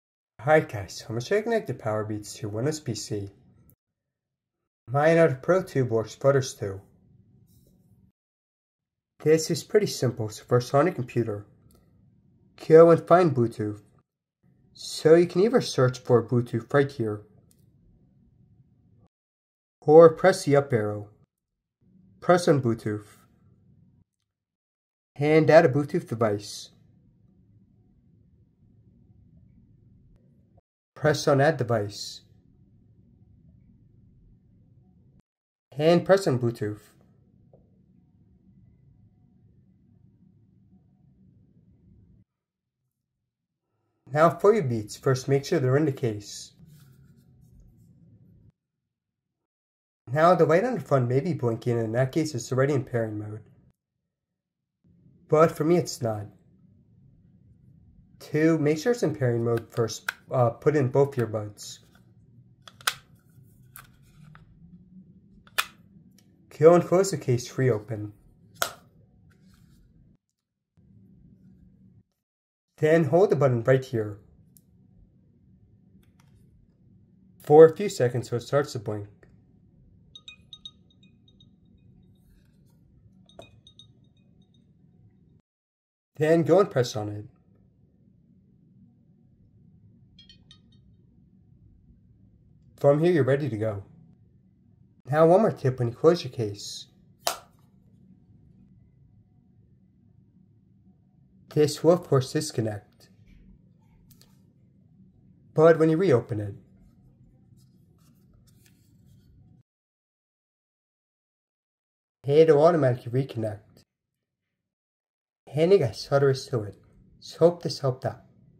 Beats Powerbeats Pro 2: How to Connect to Windows PC!
How to pair Apple Beats Powerbeats Pro 2 to a Windows 11 laptop or other computer.